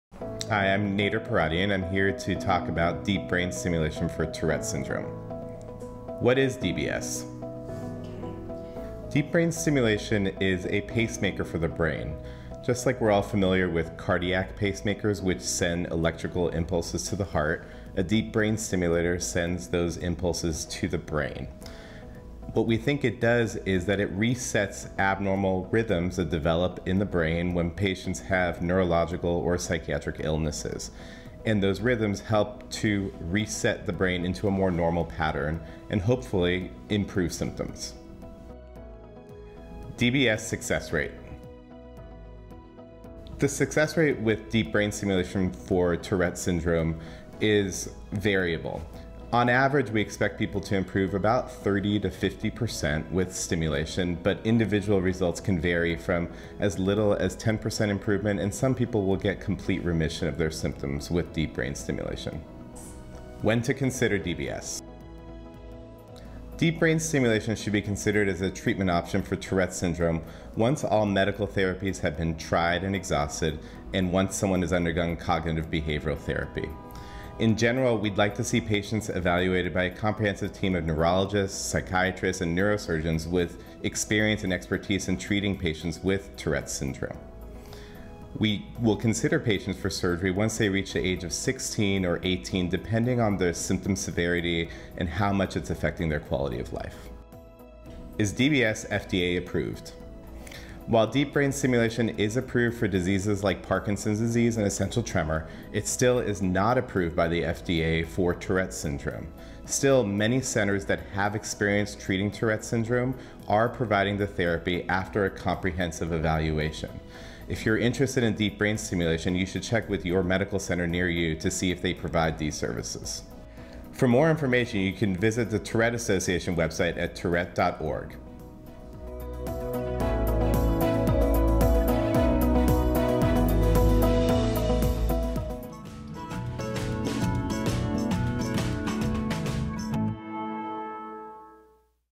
Deep Brain Stimulation
Deep Brain Stimulation: Dr. Nader Pouratian explains deep brain stimulation, a medical procedure available to some individuals with severe Tourette Syndrome.